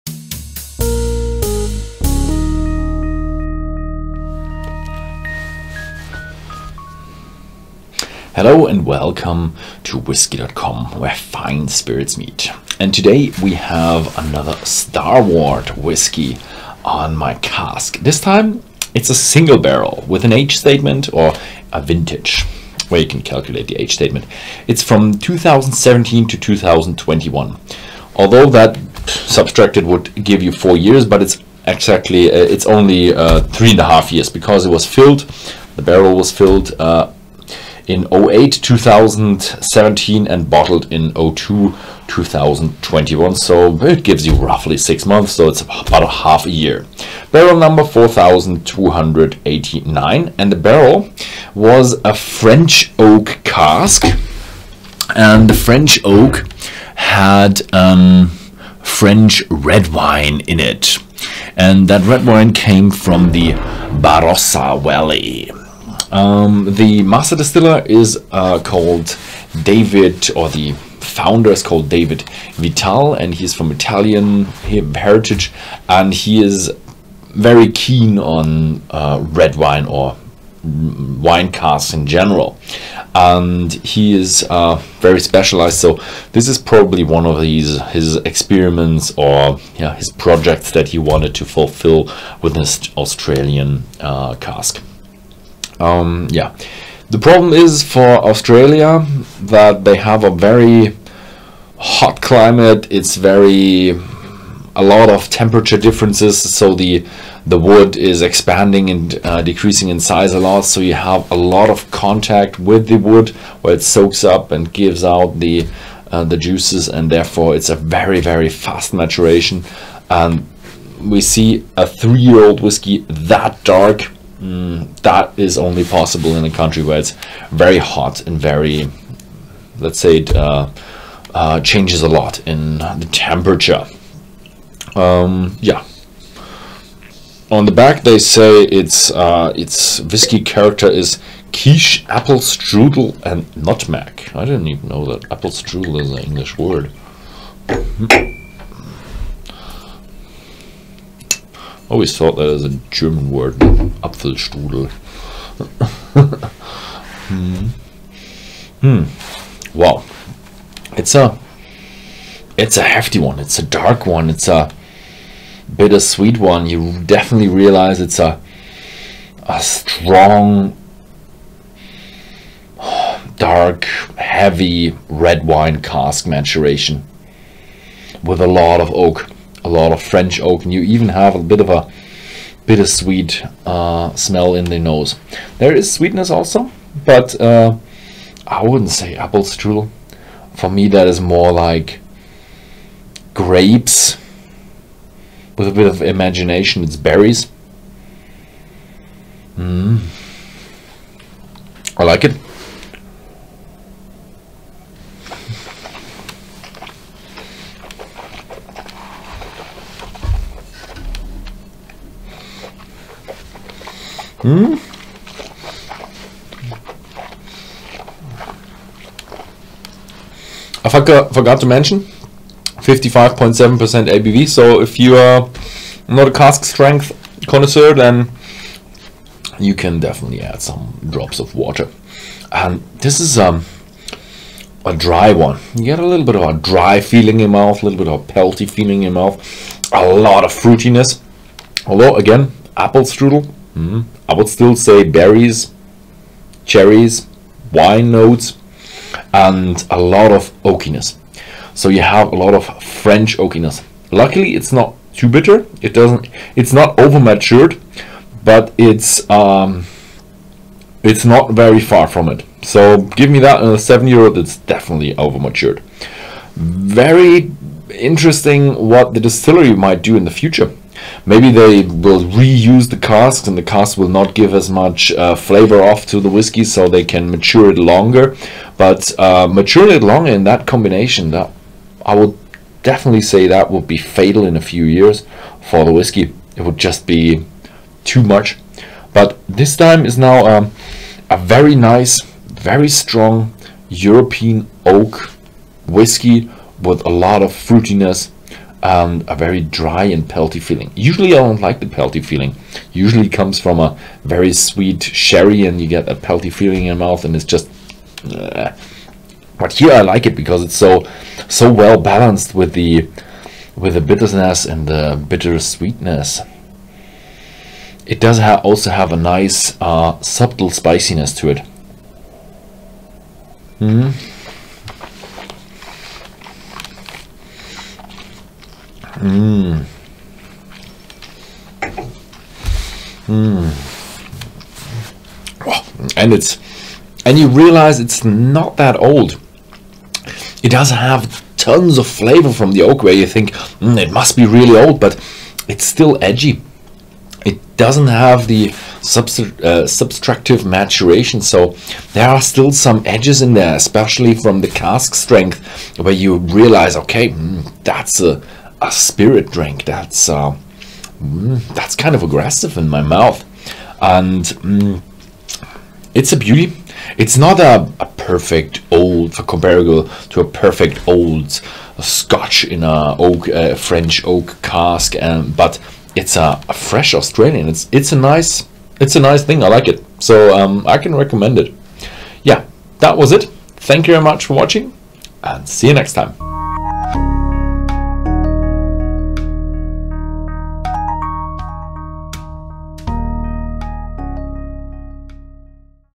Starward Single Barrel 2017/2021
Nosing 02:37
Whisky.com reviews the Starward Single Barrel 2017/2021. Für diese Starward Single Barrel Exklusivabfüllung wurde ein französisches Rotweinfass aus dem Barossa Valley verwendet. Ihren 'New Make' füllt die Brennerei mit einem niedrigen Alkoholgehalt in die Fässer, um so die Fruchtnoten und Weintannine für den Whisky zu erhalten.
Das wechselhafte Klima in Melbourne trägt seinen Teil dazu bei, innerhalb der kurzen Reifezeit, einen geschmacksintensiven Single Malt entstehen zu lassen.